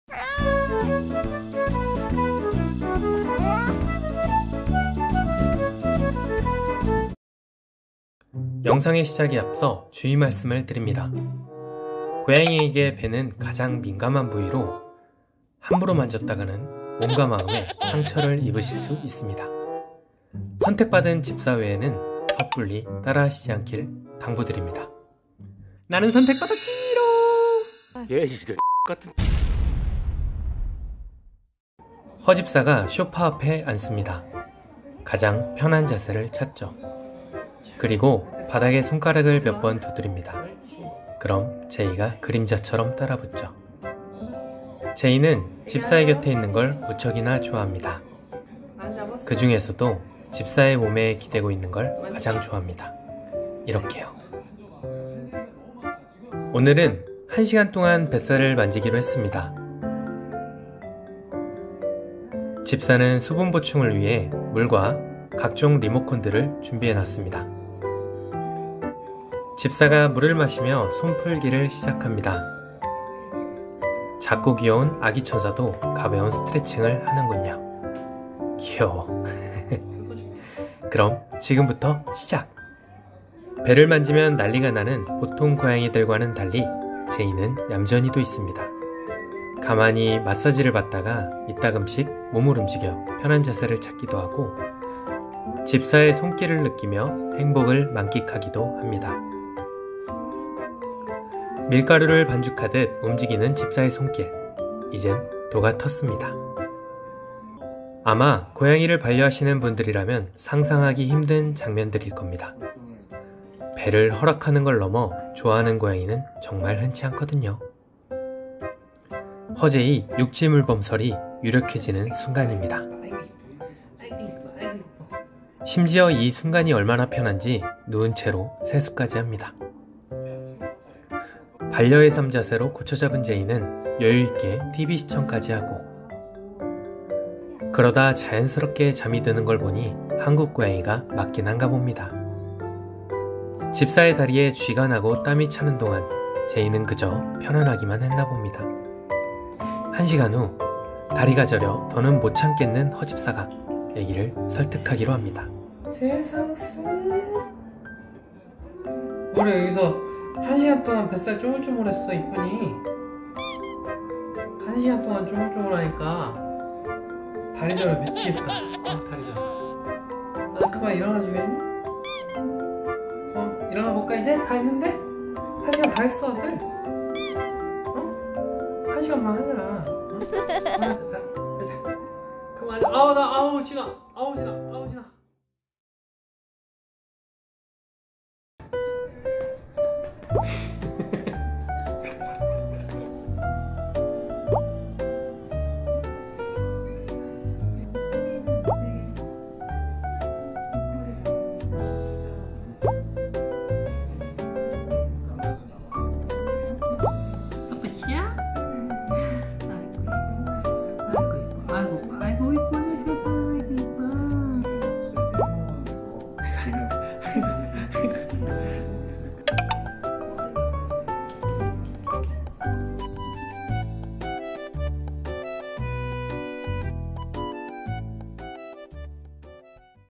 손목을 내놔야 가능하다는 고양이 뱃살만지기를 무려 한시간동안 한다고!?
쭈물쭈물 만번 넘게 함..ㅎ

- Profile -
이름 : 허제이(애칭:쁜쁜슈,허세상 등)
품종 : 코리안숏헤어(고등어태비)
성별 : 남(중성화)
나이 : 9세(2020년 기준)
생일 : 2012년 2월 22일
입양일 : 2015년 2월 17일
특이점 : 순하고 애교가 많음. 스킨십을 좋아함

(BGM)